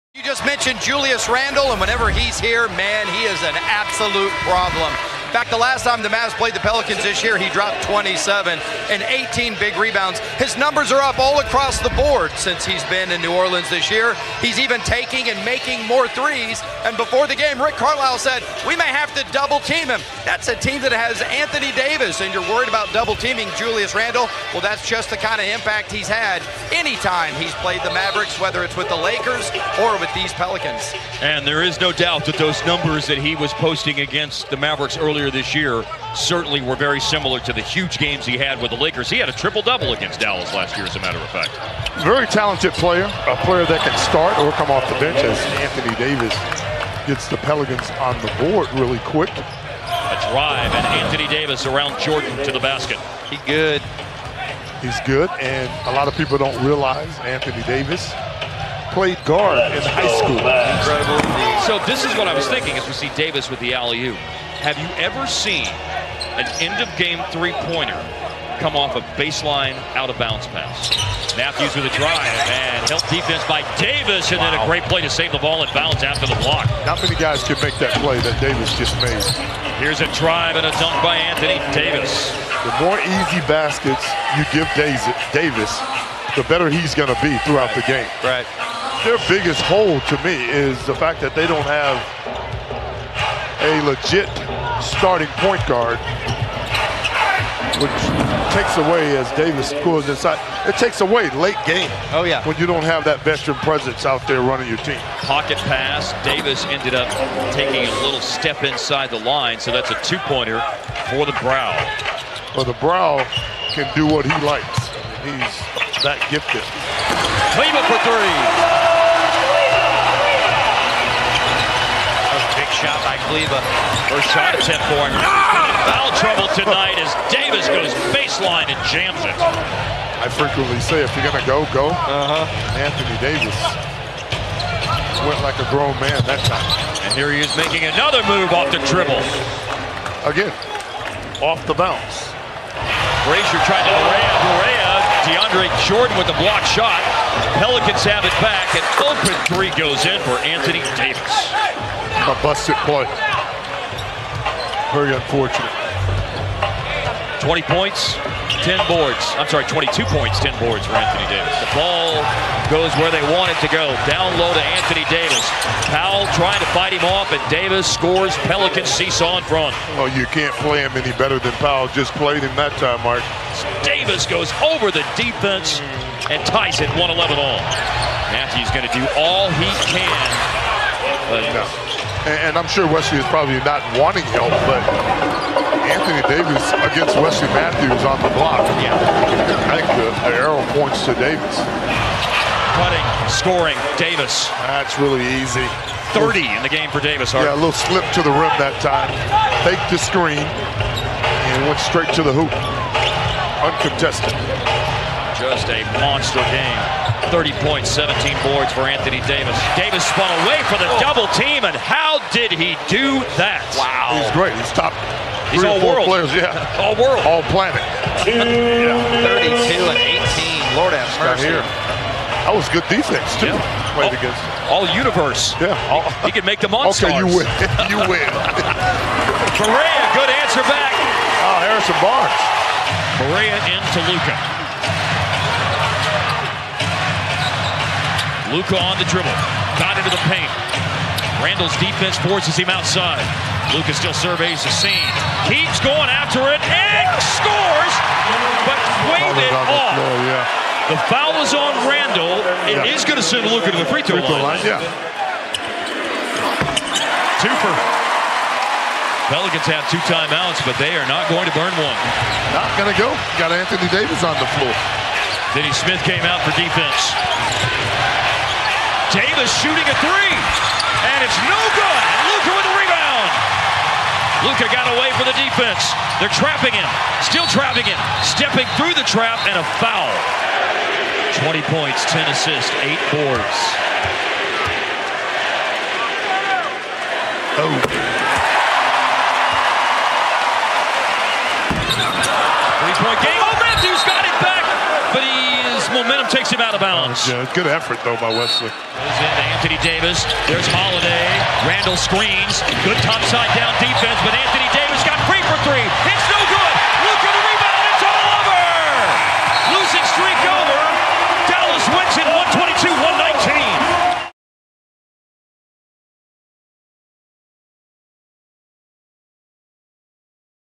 Anthony Davis Full Highlights 2018 12 26 Mavs vs Pelicans 32 Pts 18 Rebs! FreeDawkins
December 25, 2018 |  Anthony Davis Full Highlights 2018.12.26 Mavs vs Pelicans - 32 Pts, 18 Rebs! | FreeDawkins - NBA Video'